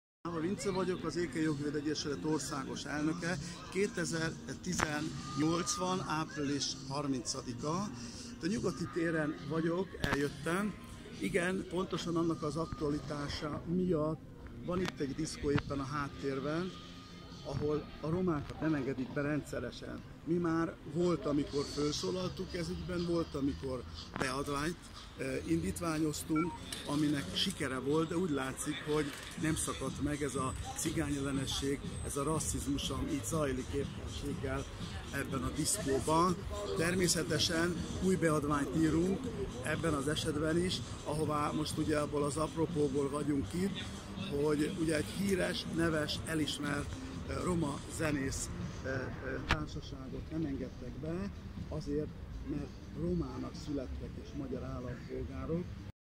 Parno Graszt Roma elismert zenekar tagjait nem engedték be a Bp nyugati téri szórakozó helyre.
Száva Vince ÉKE-IMA JOGVÉDŐ elnök beadványban tiltakozik a fajgyülölet jelenség ellen.